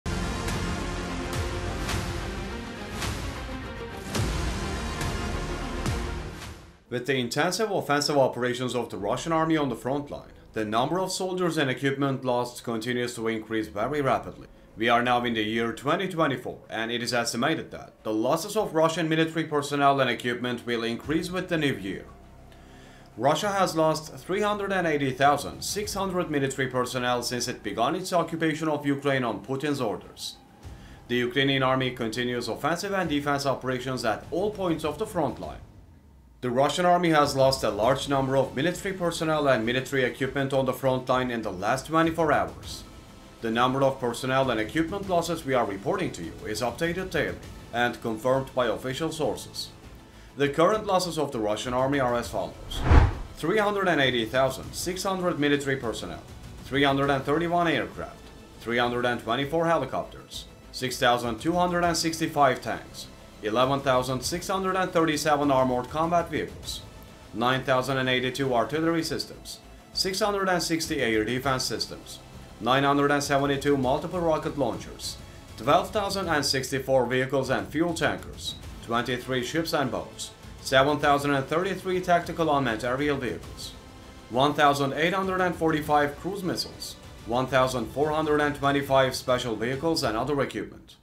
Putin's Cry for Help! Great Collapse of the Russian Army on Ukrainian Territory!
With the intensive offensive operations of the Russian army on the front line, the number of soldiers and equipment lost continues to increase very rapidly!
We are now in the year 2024, and it is estimated that the losses of Russian military personnel and equipment will increase with the new year.
Russia has lost 380,600 military personnel since it began its occupation of Ukraine on Putin's orders. The Ukrainian Army continues offensive and defence operations at all points of the front line. The Russian army has lost a large number of military personnel and military equipment on the front line in the last 24 hours.
The number of personnel and equipment losses we are reporting to you is updated daily and confirmed by official sources.
The current losses of the Russian army are as follows;